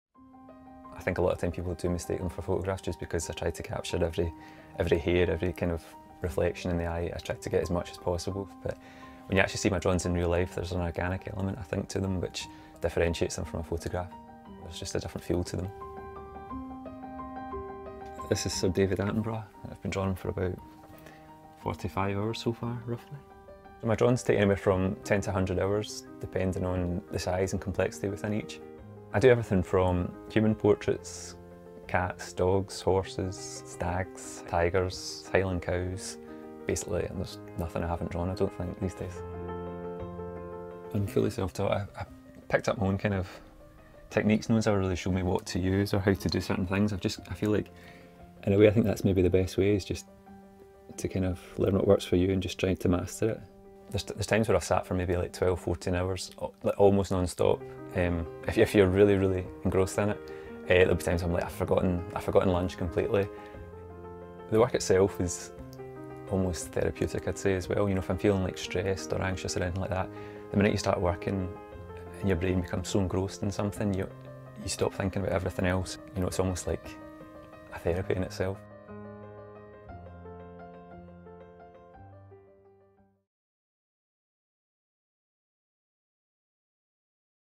‘I taught myself to draw photorealistic pictures’ - BBC URDU
سٹوئرٹ ایک ایسے فنکار ہیں جو حیرت انگیز طور پر حقیقت سے قریب پورٹریٹ بنانے میں مہارت رکھتے ہیں۔ کچھ تصاویر کو تیار کرنے میں 100 گھنٹے سے زیادہ کا وقت لگتا ہے۔ انھوں نے پورٹریٹ بنانا خود سیکھا۔ سٹوئرٹ کہتے ہیں کہ کئی بار لوگ ان کے کام کو غلطی سے کیمرے سے اتاری گئی تصاویر سمجھ بیٹھتے ہیں۔